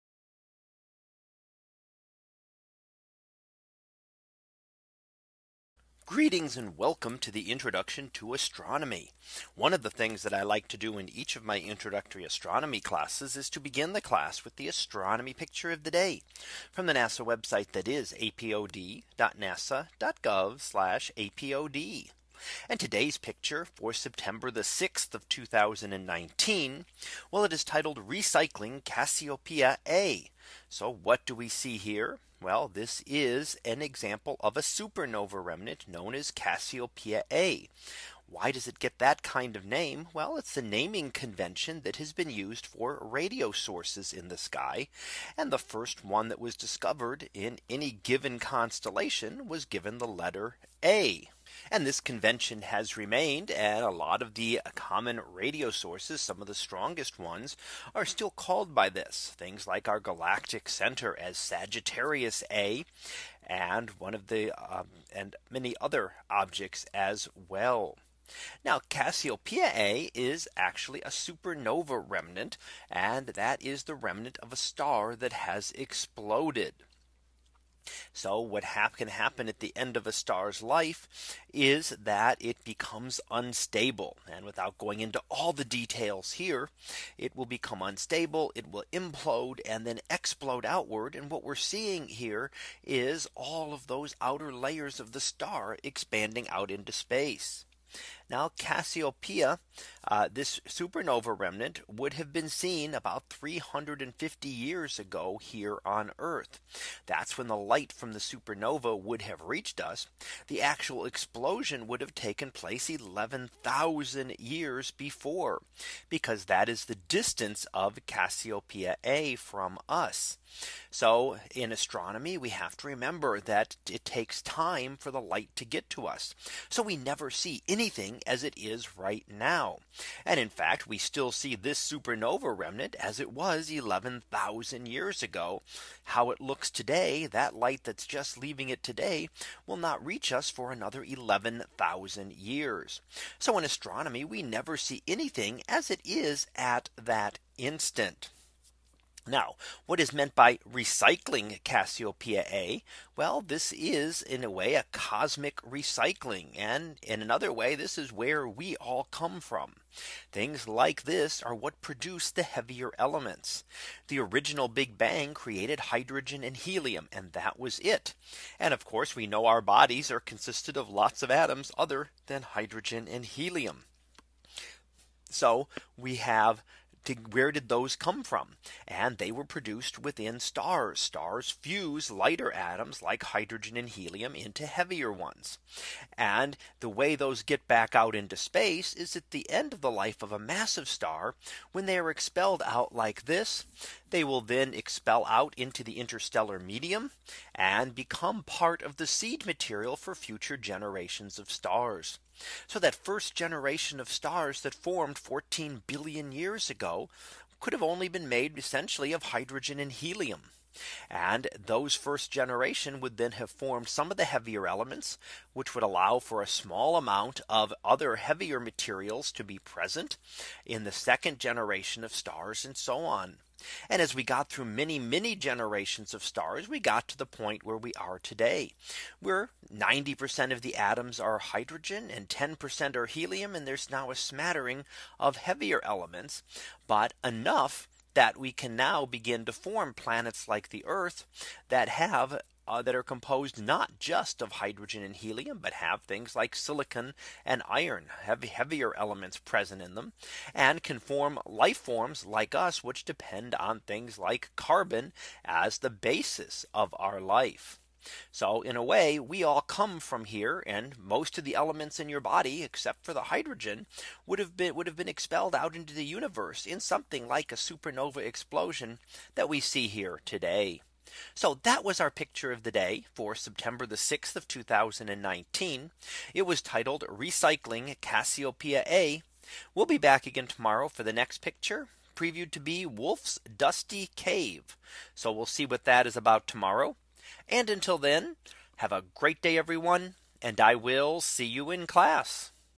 2019 September 06 - Recycling Cassiopeia A
In today's image, we look at the supernova remnant known as Cassiopeia A. Supernovae such as this one are how the heavy elements (anything other than hydrogen or helium!) are returned to the interstellar medium to become a part of the next generation of stars.